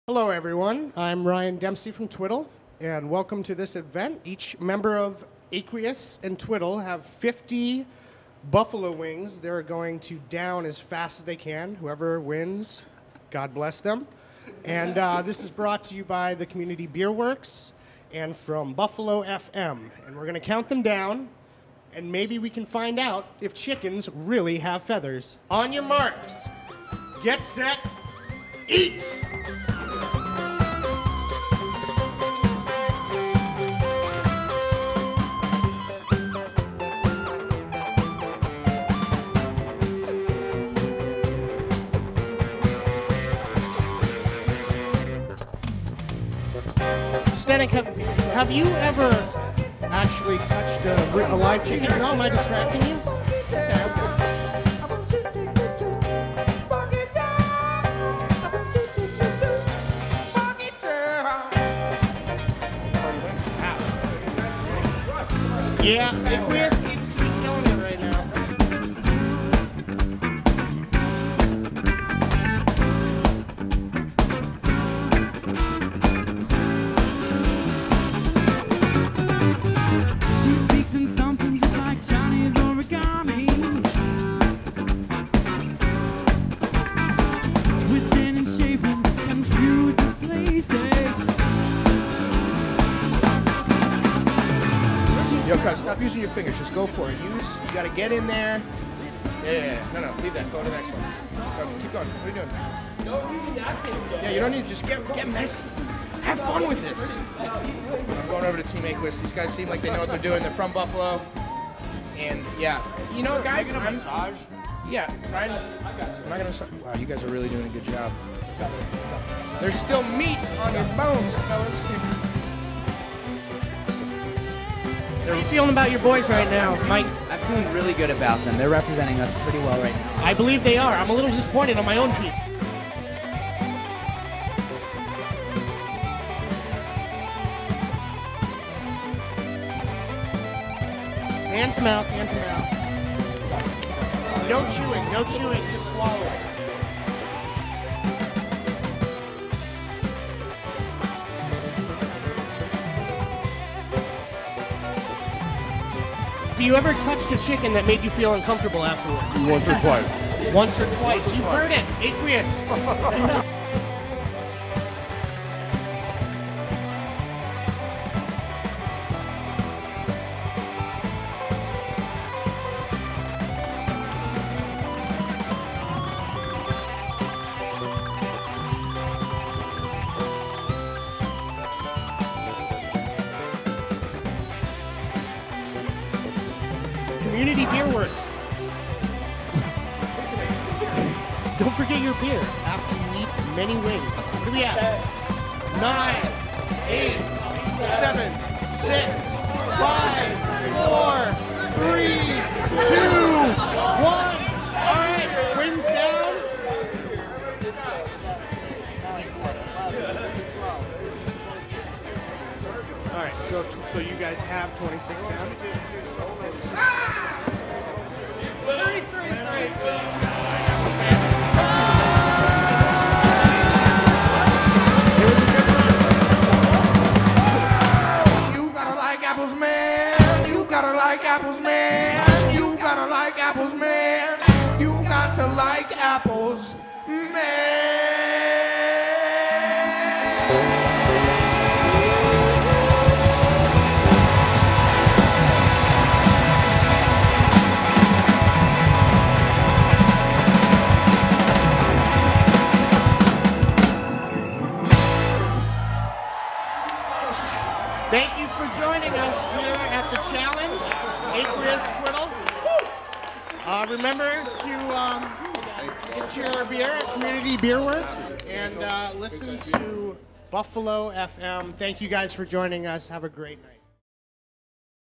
Twiddle vs Aqueous Buffalo.fm Chicken Wing Throwdown
Buffalo.fm and Community Beer Works offered up a challenge between Twiddle and Aqueous during their two night run at Town Ballroom on March 4th and 5th.